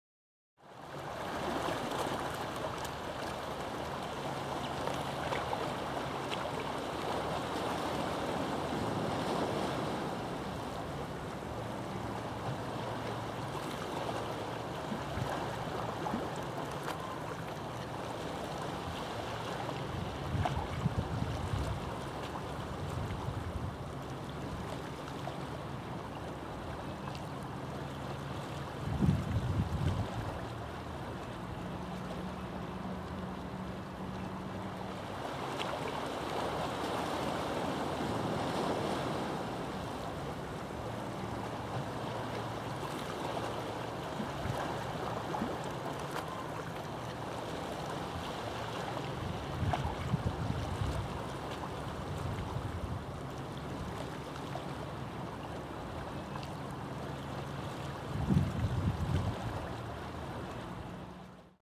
Purple Sandpiper
some 30-50 Purple Sandpipers over winter at Jubilie Pool, Penzance, Cornwall. and are best looked for on a rising tide.